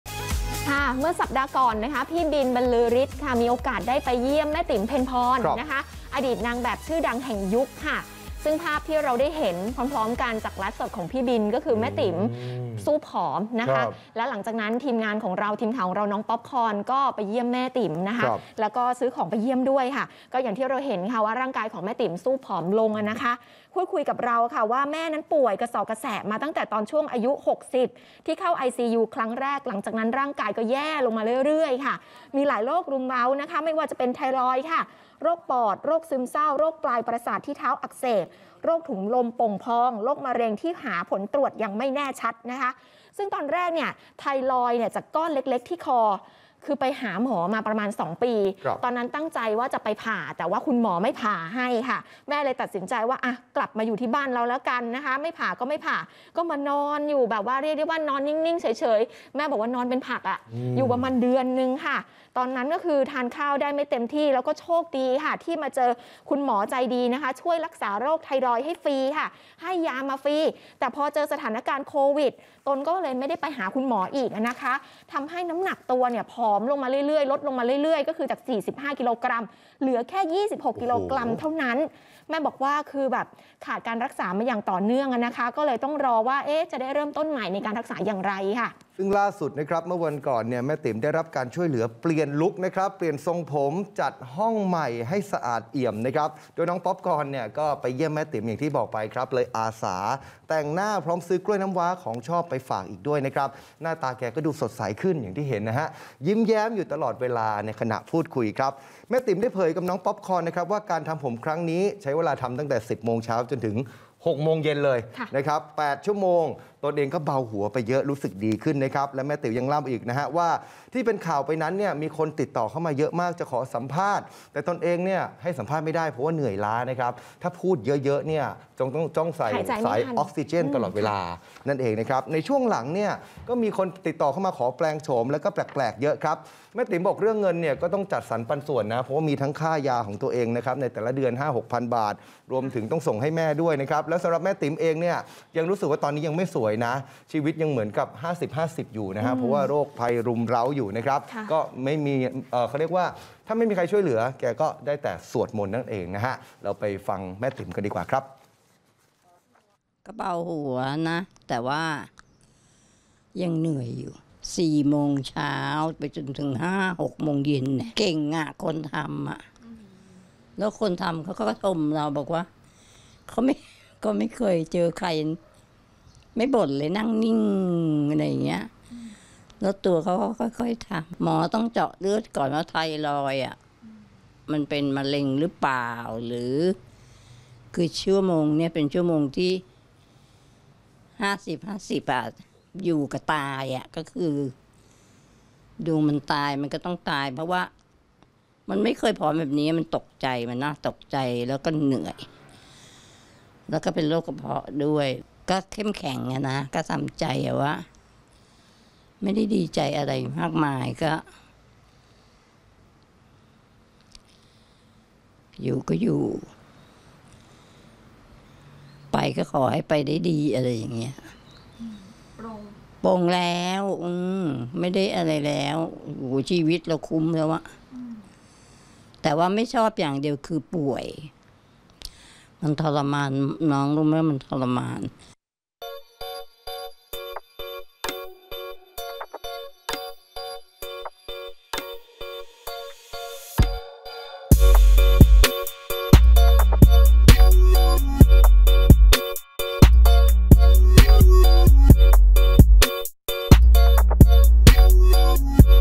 "ติ๋ม เพ็ญพร" ทำใจยอมรับความตาย หลังป่วยโรครุมเร้า | Apop Today
หลังจากที่เมื่อสัปดาห์ก่อน "บิณฑ์ บรรลือฤทธ์" ได้ไลฟ์สดเผยสภาพ "ติ๋ม เพ็ญพร ไพทูรย์" ที่นอนผอมแห้ง ผมฟูอยู่ภายในห้องพักเพียงคนเดียว ต่อมาทีมข่าวอมรินทร์ได้ลงพื้นที่จึงทราบว่าแม่ติ๋มป่วยหลายโรคด้วยกัน เพราะอาติ๋มไม่มีแรงทำอะไรได้แค่ลุกนั่งๆนอนๆ หลังจากที่ตนเองป่วยกระเสาะกระแสะมาตั้งแต่อายุ 60 ปีที่เข้า ICU ครั้งแรก หลังจากนั้นร่างกายก็แย่ลงเรื่อยๆ   ตอนนี้ป่วยอยู่ 6 โรคด้วยกัน มีโรคไทรอยด์ โรคปอด โรคซึมเศร้า โรคปลายประสาทที่เท้าอักเสบ โรคถุงลมโป่งพอง และโรคมะเร็งที่ผลตรวจยังไม่แน่ชัด

 ในเริ่มแรกนั้นตนเองเป็นไทรอยด์จากก้อนเล็กๆที่คอ หาหมออยู่ 2 ปี ทานยามาเรื่อยๆ ก้อนไทรอยด์ก็ใหญ่ขึ้นมาทันที ตนเองก็ตกใจสงสัยว่าทำไมตอนรู้ว่าเป็นอะไรคุณหมอไม่รีบเอาก้อนเนื้อออกเพราะเป็นมะเร็งแล้ว จากนั้นหมอก็ส่งตัวไปโรงพยาบาลอีกที่นึง เพื่อผ่าตัด ก็โดนคุณหมอไล่กลับมาบอกว่าผ่าไม่ได้ ตนเองก็งง มิหนำซ้ำหมอยังต่อว่าอีกผ่าไปก็นอนรอความตายอย่างเดียว

จากเหตุการณ์ครั้งนั้นตนเองเลยตัดสินใจไม่ผ่า เลยกลับมาบ้านนอนเป็นผักไป 1 เดือนเต็มที่ตนเองไม่ได้กินข้าวเลย โชคดีที่เจอหมอดีตนเองเลยได้รักษาไทรอยด์ฟรี ให้ยาฟรี แต่พอโควิดมาก็ไม่ได้ไป เลยทำให้ตนเองผอมลงจากน้ำหนัก 45 กิโลกรัม เหลือ 26 กิโลกรัม เพราะขาดการรักษาอย่างต่อเนื่อง ตอนนี้เลยทำให้ต้องรอเริ่มต้นใหม่  ขอบคุณทุกกำลังใจที่หยิบยื่นความช่วยเหลือมาให้